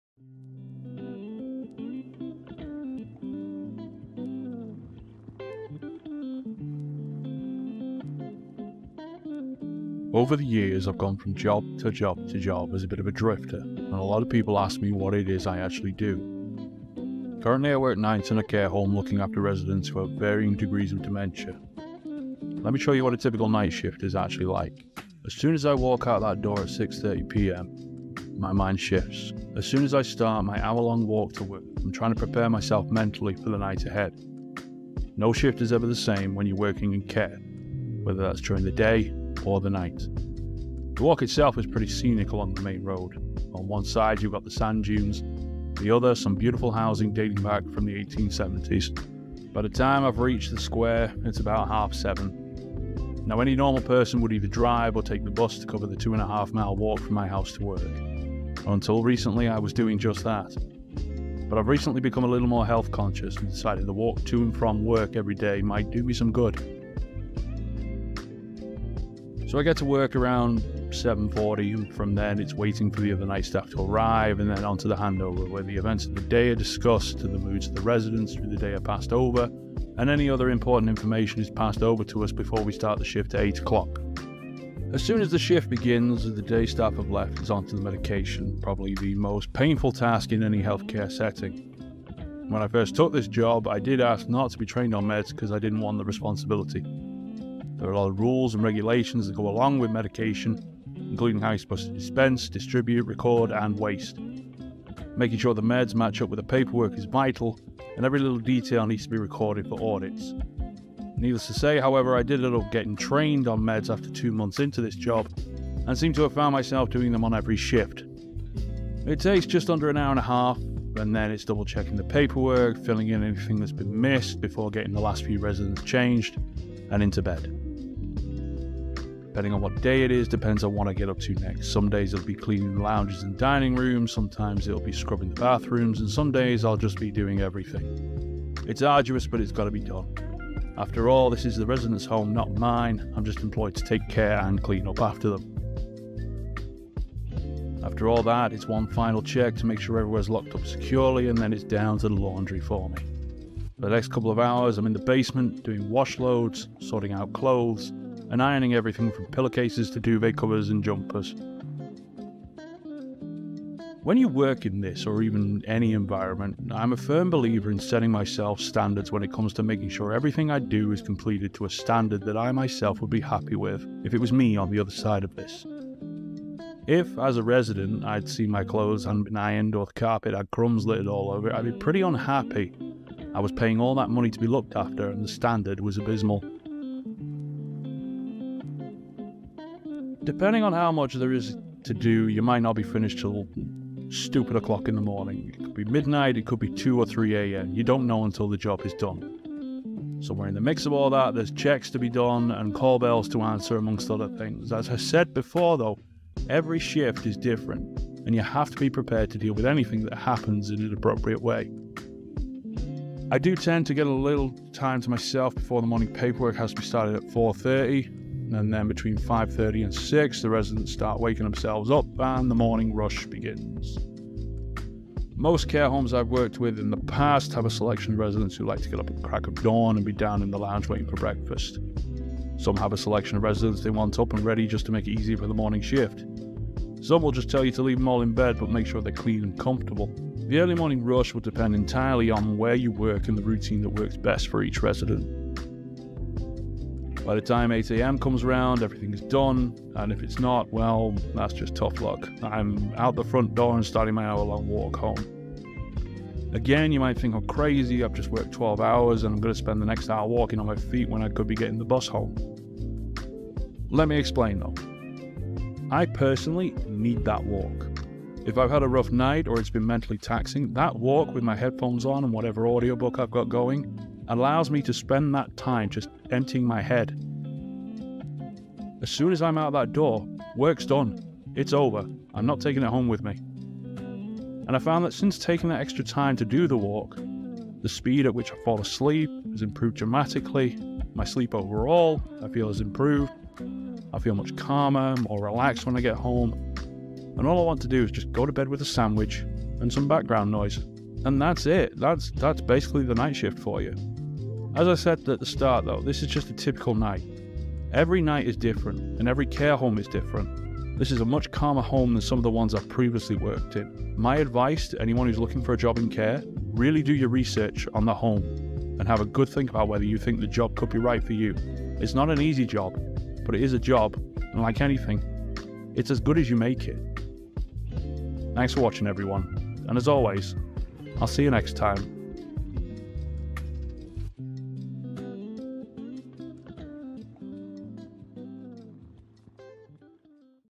A Night in the Life of a Night Shift Healthcare Assistant
In this video, I'm sharing insights from my night shift as a Healthcare Assistant. From dealing with tricky patients to staying calm in the face of chaos, I'll share my experience of working the late hours.

If you're interested in a career in healthcare, then this video is for you! In it, you'll learn about the challenges and rewards of working late hours, and you'll see what it's like to work in a busy care home setting. If you're looking for an insight into what it's like working in the Healthcare Sector, then this is the video for you!